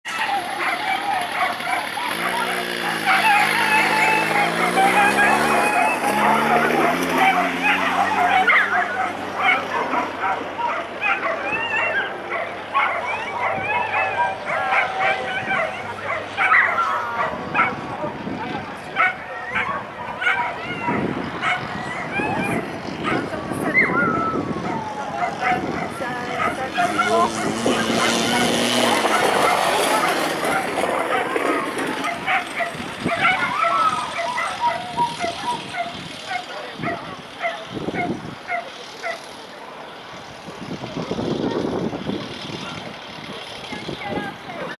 Whippets - Cynodrome Perpignan
Whippets au Cynodrome de Perpignan lors de l'ENC du 08 Février 2015